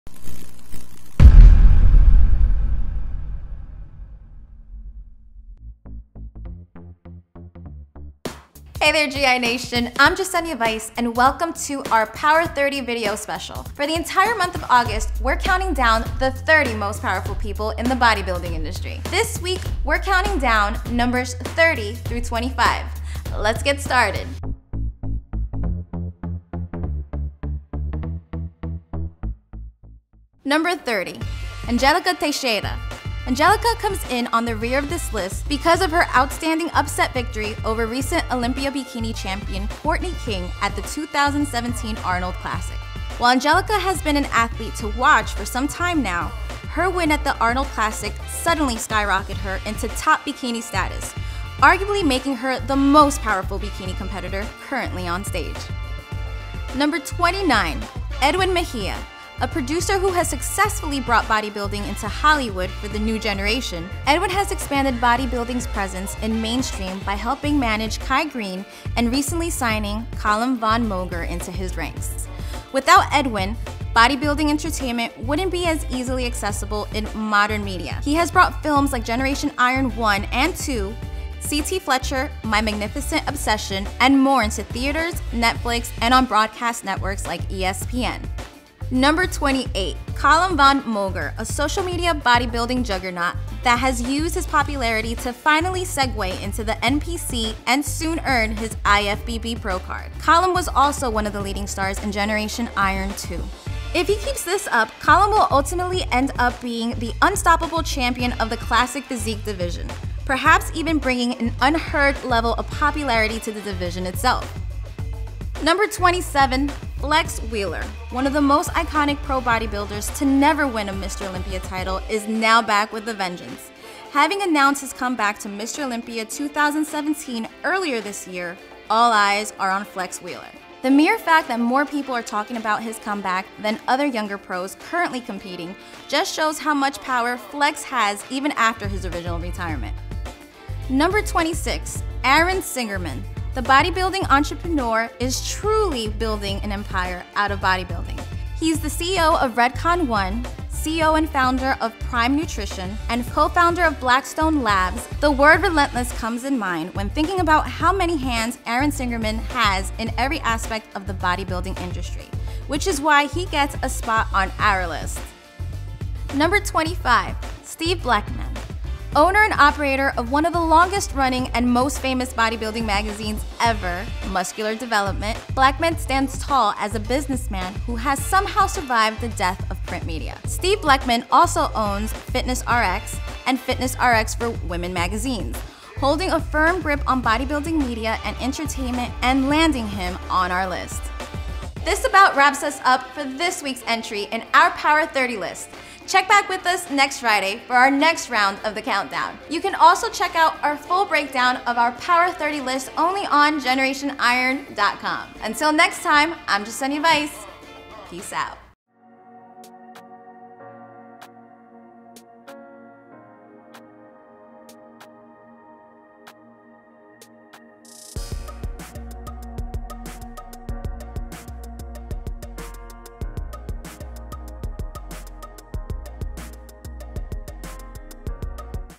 Power 30: The Most Powerful People In Bodybuilding Today | 30-25
POWER 30 - is our annual attempt to create a ranking through in depth discussions amongst our editorial staff of the 30 most powerful athletes, CEOs, and personalities that have made an impact on bodybuilding this year. We'll be counting down in five installments every Friday.

You've seen the "Best Of" lists before in bodybuilding - but these mostly consist of the best athletes of past and present. While fun they all start to look the same year after year. That's why we are now counting down the most powerful people in the industry both on and off the stage. That includes CEOs, coaches, gurus, athletes, and social media influencers. Today we're kicking off the list with numbers 30 through 25. Who made the top 30? Find out in the video above.